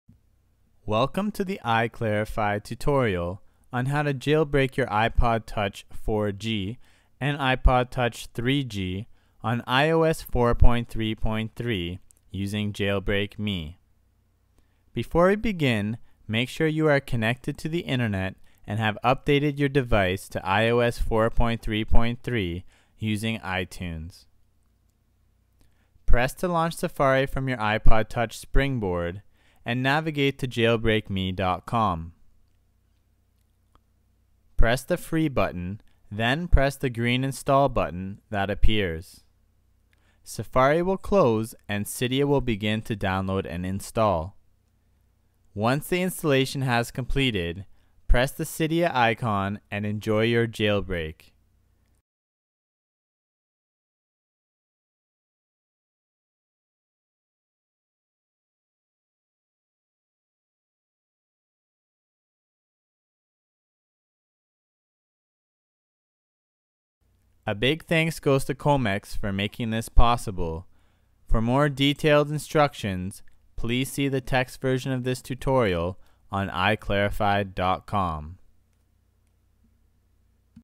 How to Jailbreak Your iPod Touch 4G, iPod Touch 3G on iOS 4.3.3 Using JailbreakMe
iClarified instructions on how to jailbreak your iPod touch 4G, iPod touch 3G on iOS 4.3.3 using JailbreakMe. You can find more detailed instructions in the text version of this tutorial found